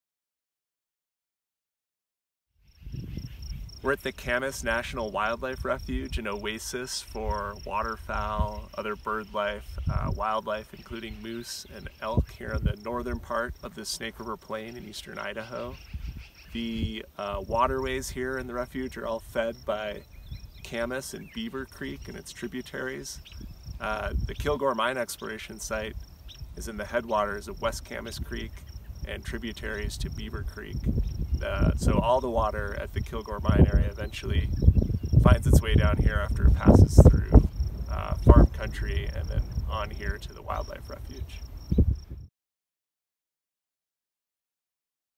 Visit to Camas National Wildlife Refuge
Advocates for the West Staff Attorney Bryan Hurlbutt visits Camas National Widlife Refuge, upstream of the Kilgore project site in the Centennial Mountains of eastern Idaho. We're fighting to protect wildlife, water quality, and rural character in this area along the "High Divide," linking Yellowstone to wilderness and roadless areas vital to migrating grizzly bears and other sensitive species.